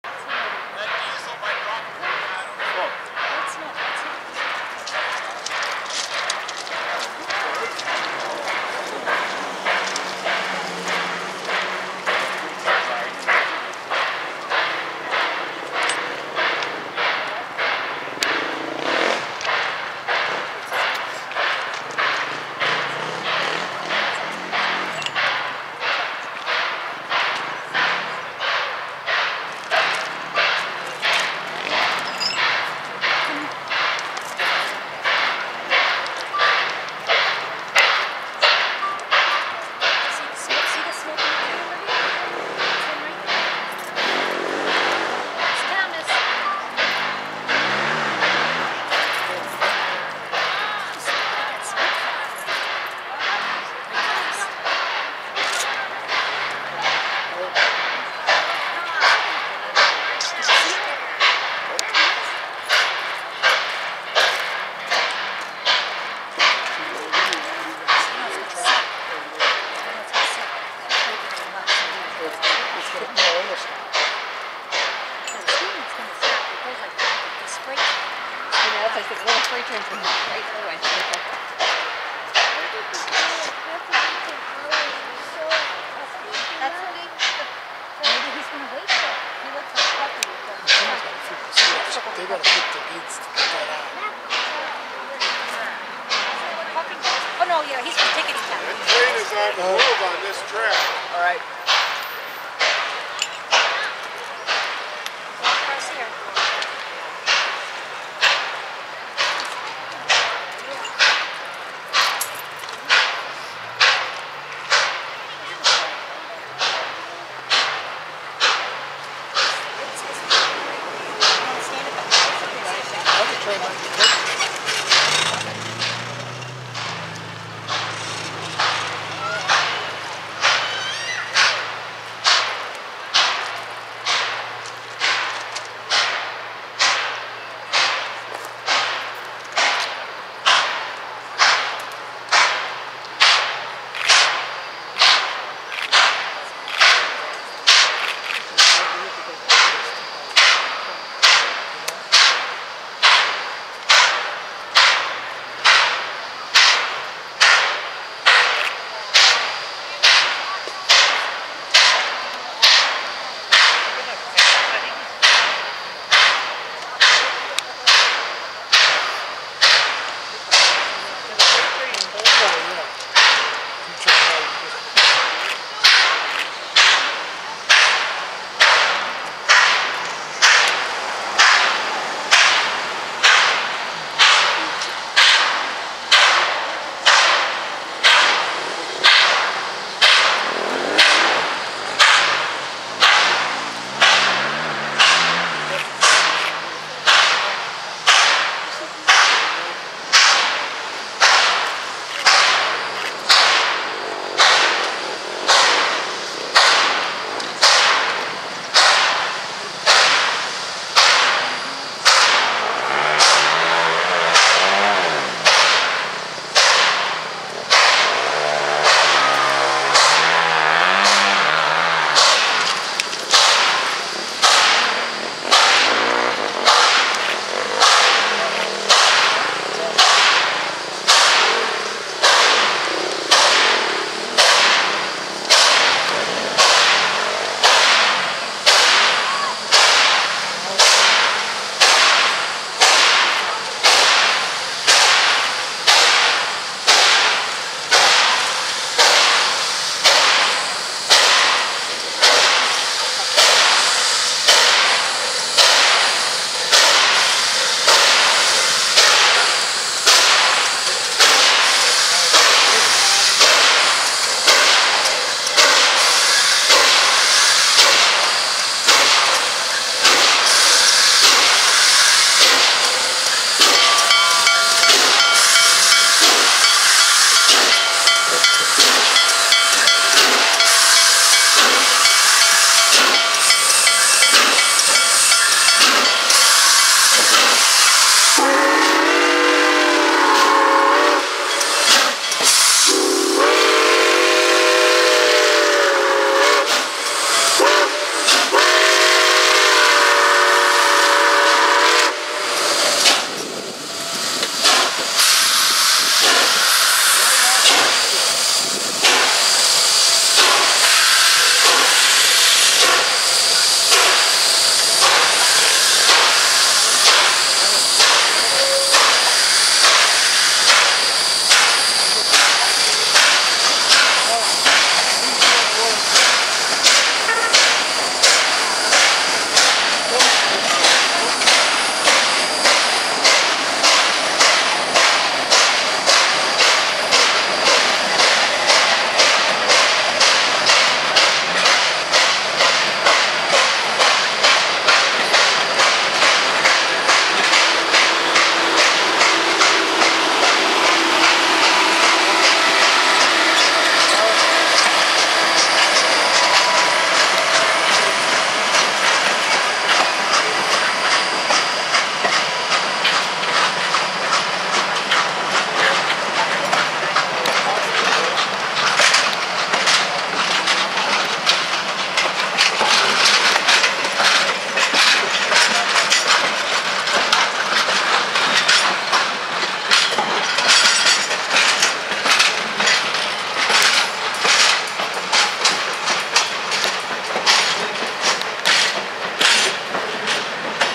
NKP 765 Departing Fairlane Yard
Completing the first leg of its excursion, 765 chugs down the eastern leg of the wye, preparing to return to Rockport Yard.